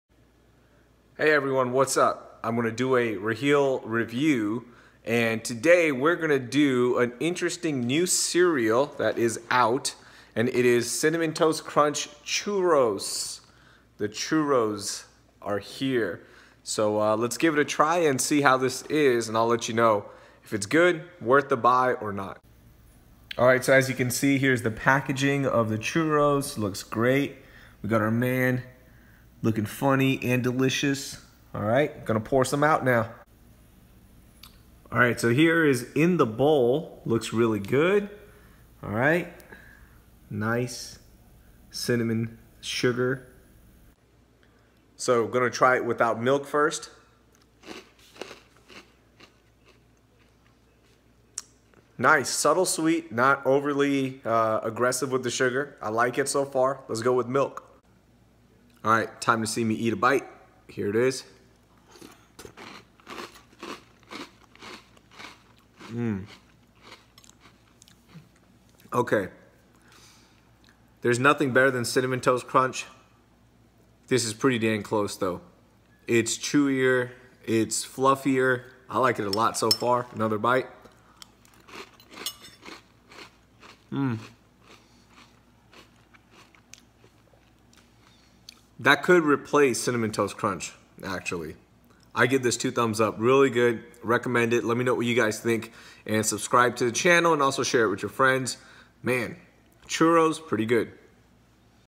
RaheelReviews Cinnamon Toast Crunch Churros
Raheel takes a bite of deliciousness with a RaheelReviews Cinnamon Toast Crunch Churros